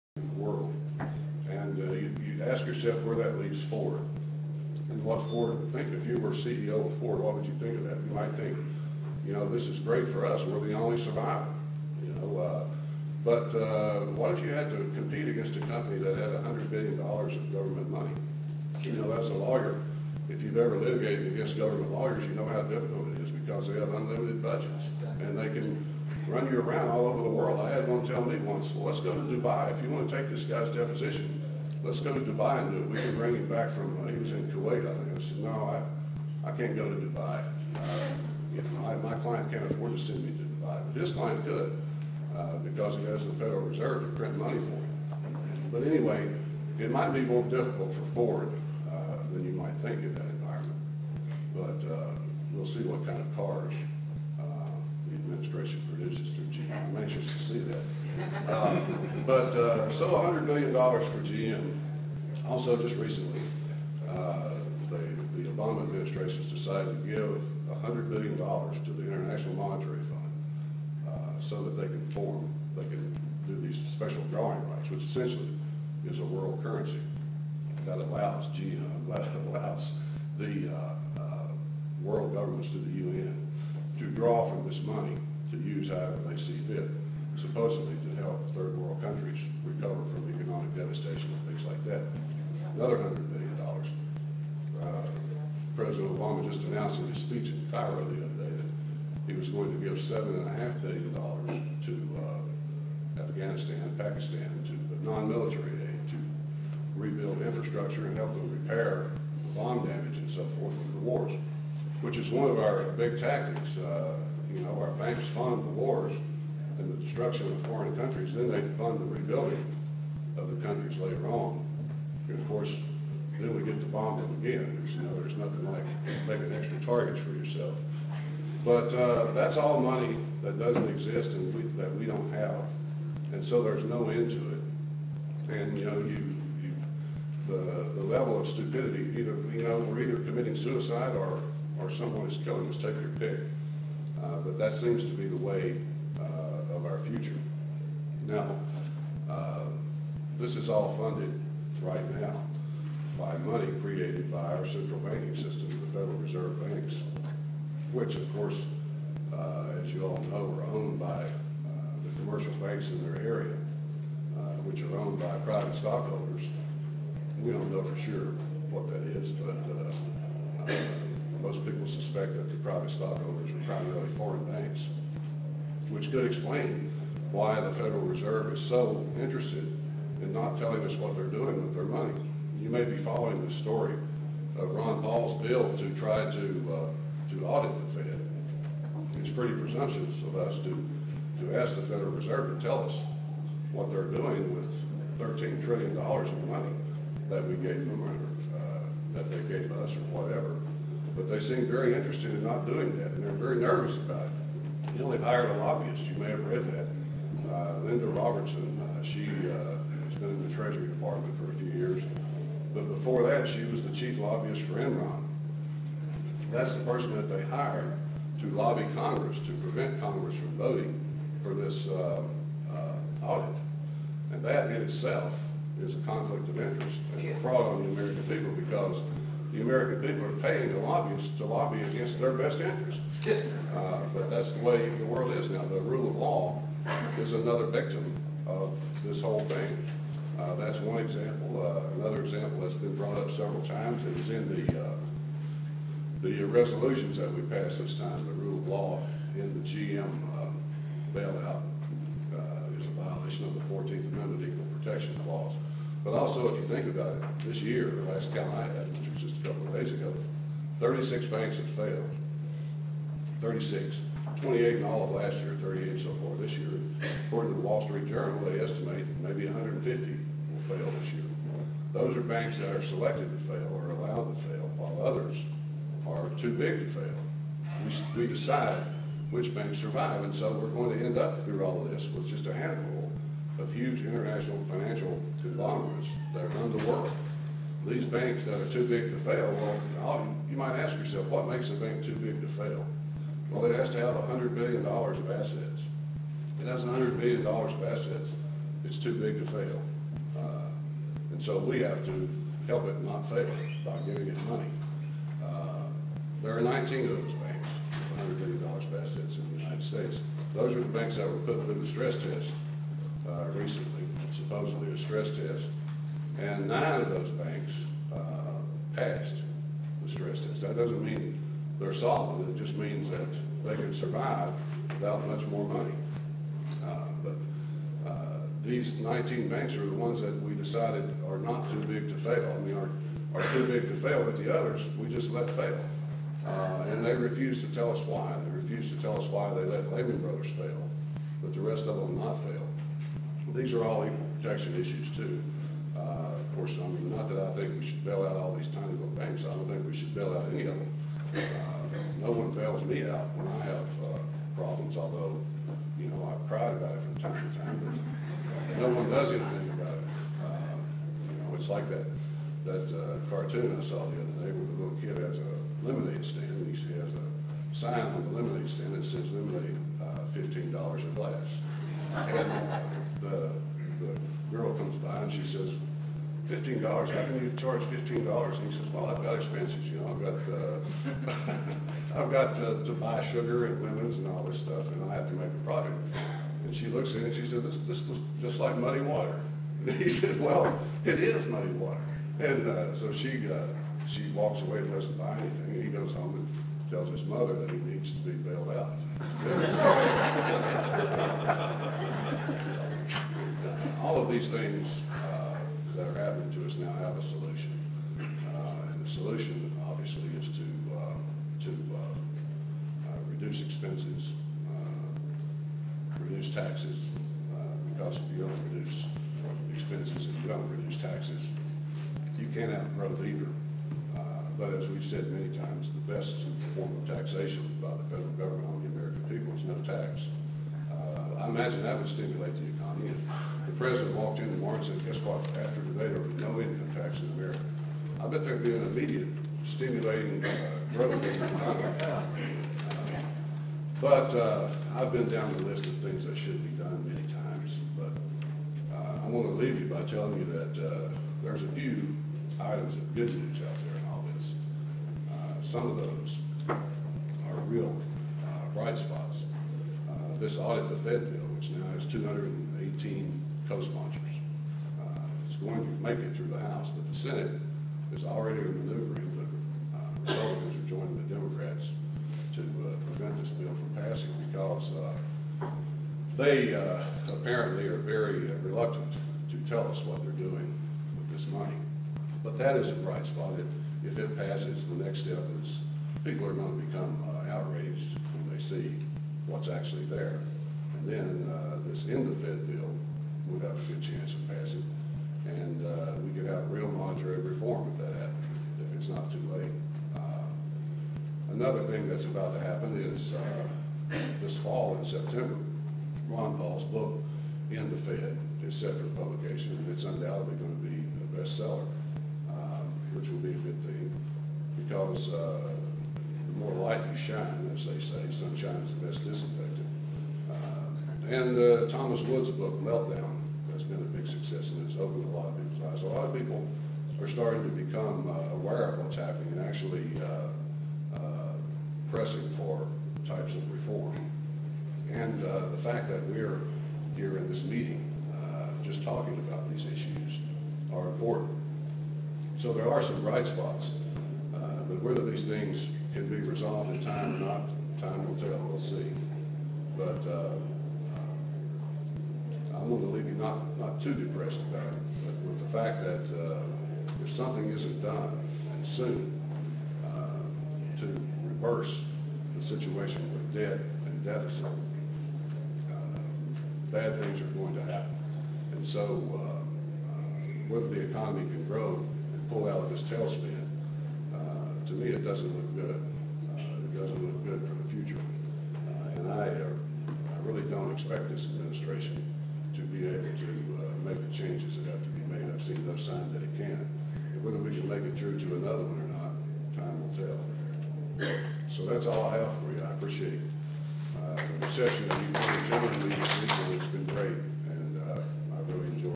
Constitution Party Speaker - Darrell Castle Part 3
Constitution Party Vice Chairman Darrell Castle spoke at the Constitution Party National Committee Meeting which was held in Newark, New Jersey on June 13, 2009. Darrell Castle also ran for Vice President of the United States in 2008. Mr. Castle who also happens to be a bankruptcy attourney spoke about the Federal Reserve, fiat money, deficit spending and the current economic recession. This is the third part of three segments.